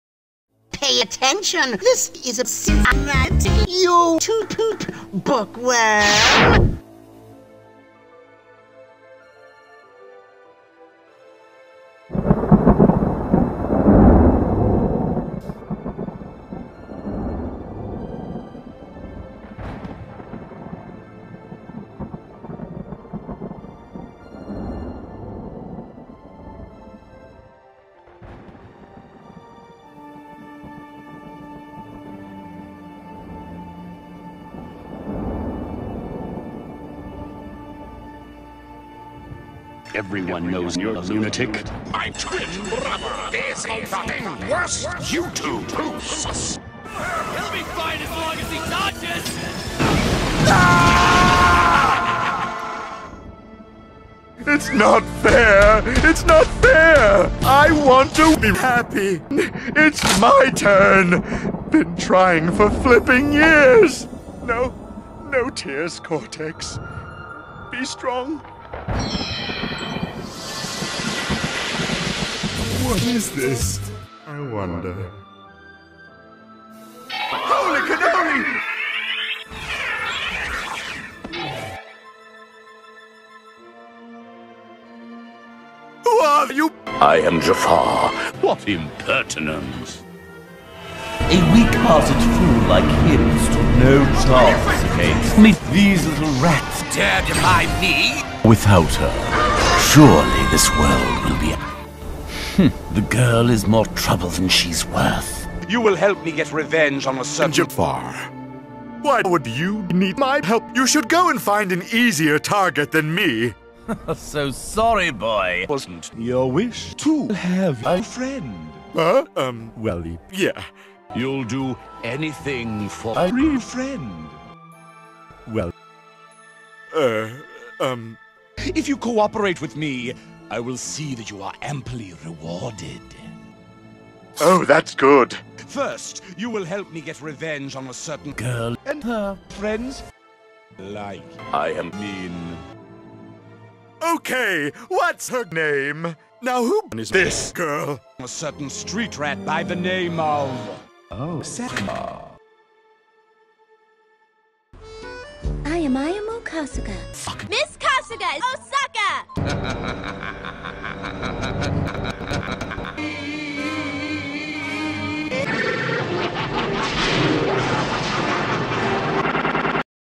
YTP collab entry: Cortex Frees a Vengeful Sorcerer
My entry for Jimmy Davis's collaboration! Hope this can make the cut!

For this YTP, I like to show how I do crossovers and stories in my YTPs. I use typical elements like reversing, stutter loops, earrape, etc, But I've been working on chrome keying, masking, and sentence mixing to tell a story with the serial nature of YTPs. And yes while this is a preview of OFFD, hell, its the first scene in the prologue act, I figured this would be my best way to introduce myself as YTPer... hopefully this is good enough